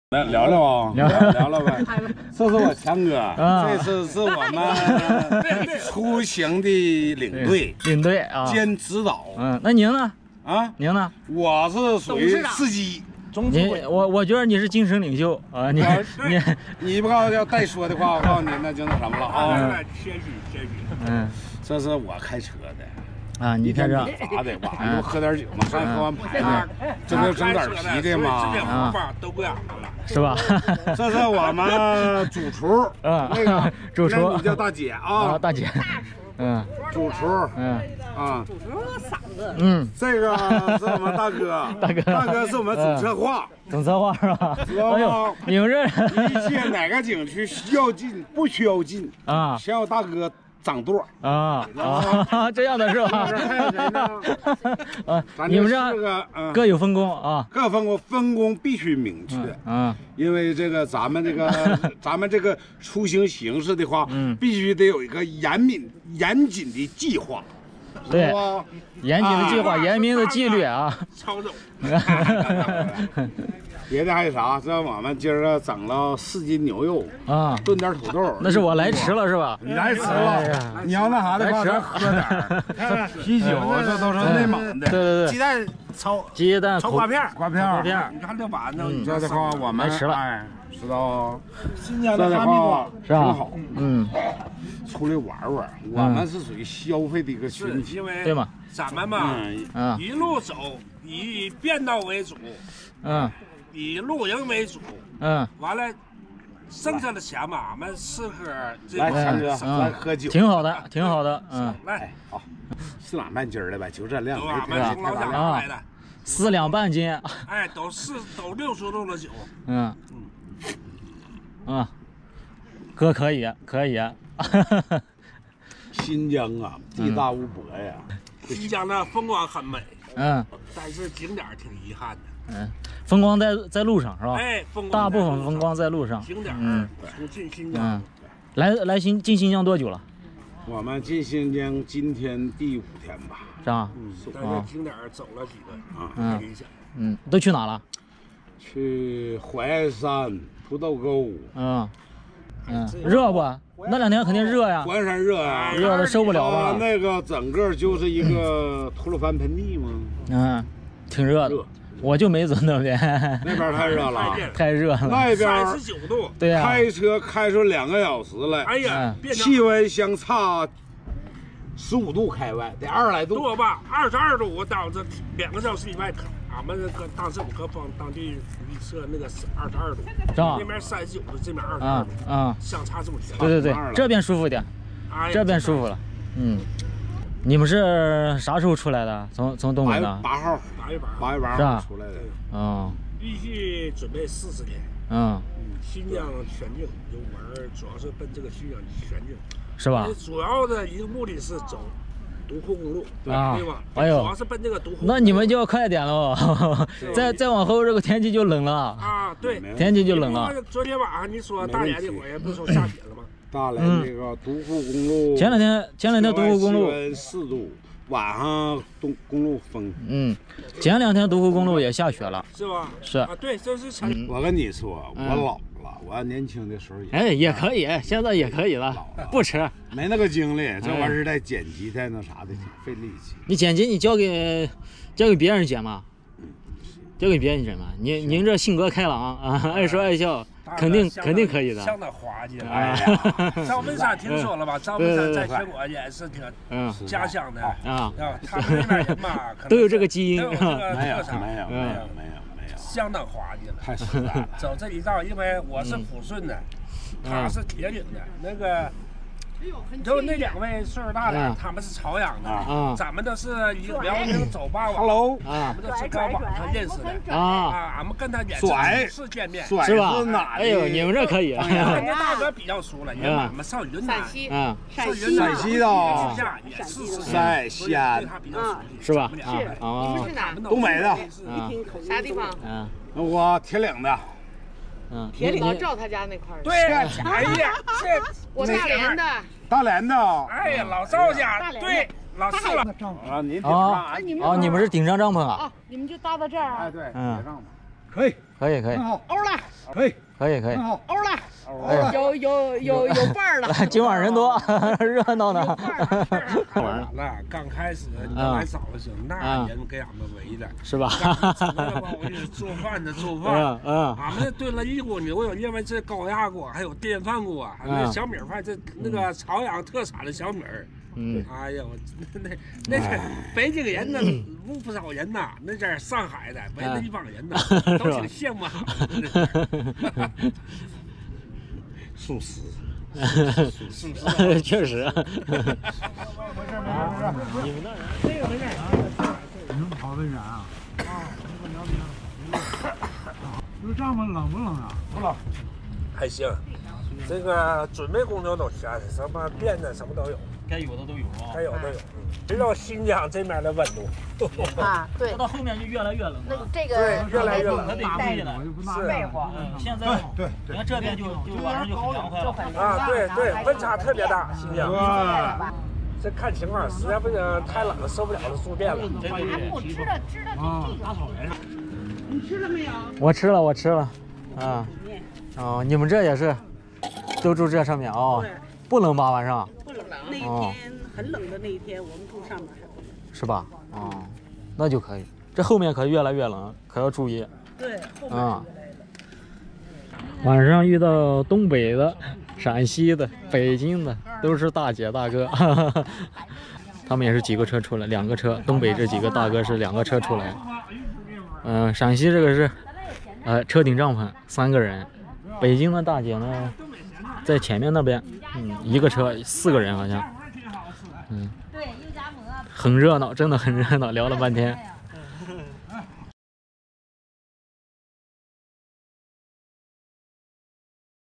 031轿车+帐篷 中国现在好多人这么玩 还遇到东北大哥 特开心 准备玩遍新疆
真的热闹